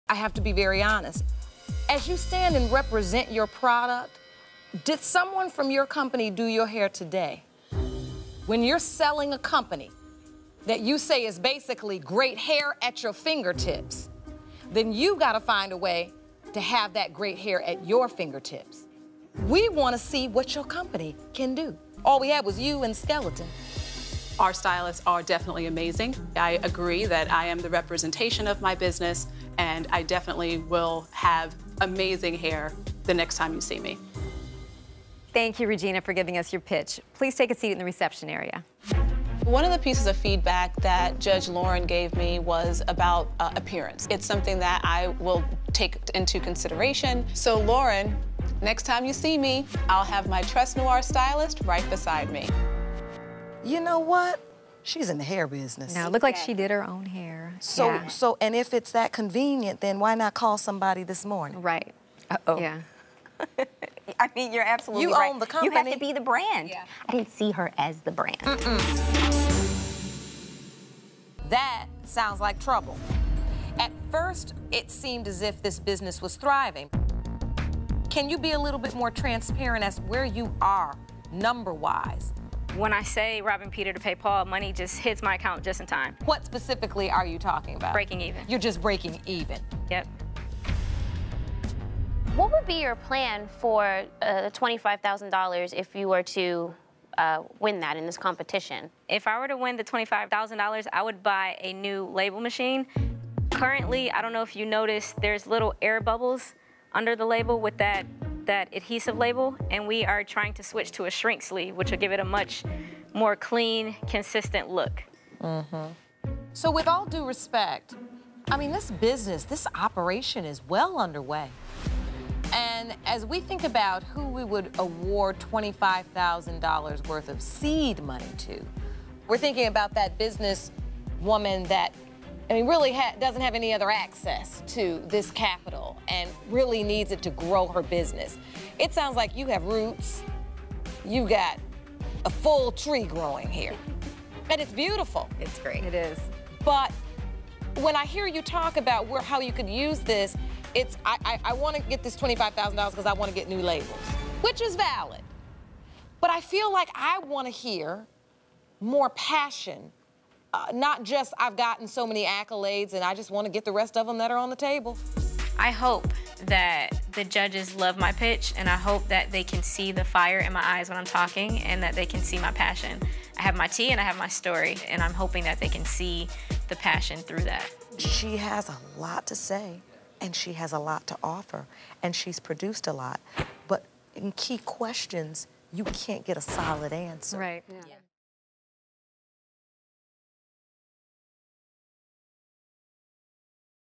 Reflecting Your Product Is Important
Some lack the passion for their products, while others can't give solid answers to what the judges are asking. Who will get a pass after the pitch?